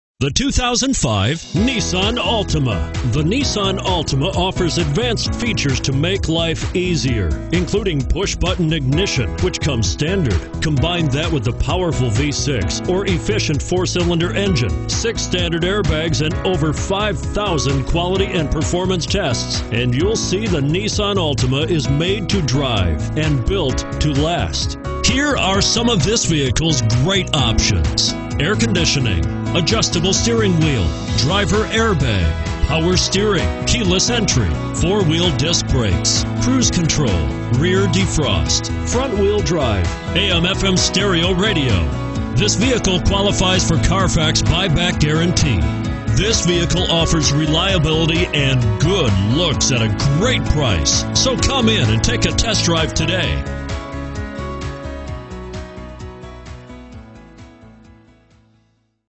2005 Nissan Altima Garland TX F0347A
2005 Nissan Altima 2.5 S

Prestige Garland
3601 S. Shiloh Road

In this 2005 Nissan Altima 2.5 S, enjoy every drive with prime features like tachometer and power windows. This 4 dr sedan scored a crash test safety rating of 4 out of 5 stars. According to a review from New Car Test Drive, Audio controls are mounted high on the center stack for easy access. Is this the vehicle you've been waiting for? Come on down and find out for yourself!